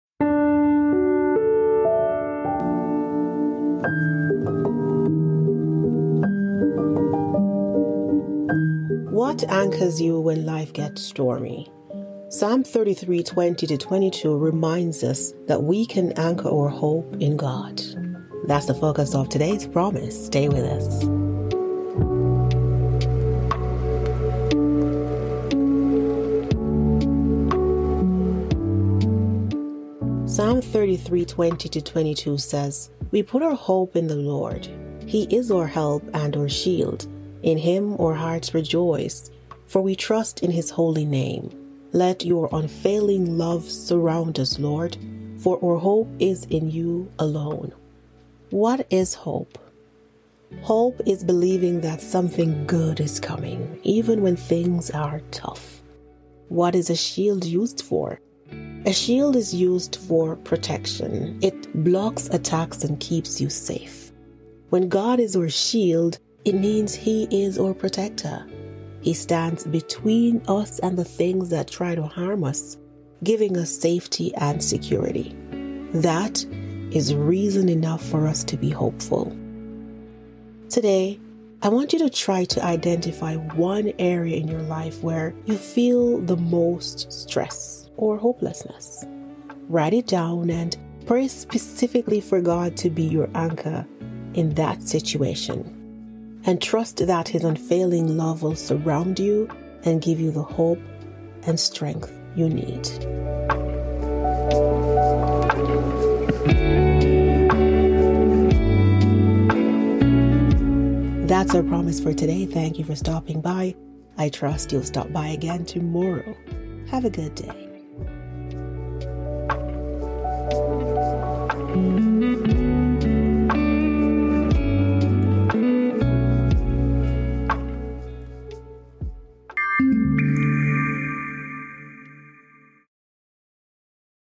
God Will Be Your Anchor In The Storm - September 27, 2024
What anchors you when life gets stormy? Psalm 33:20-22 reminds us that we can anchor our hope in God.

TODAY'S CHALLENGE: Identify one area where you feel the most stress or hopelessness. Write it down and pray specifically for God to be your anchor in that situation.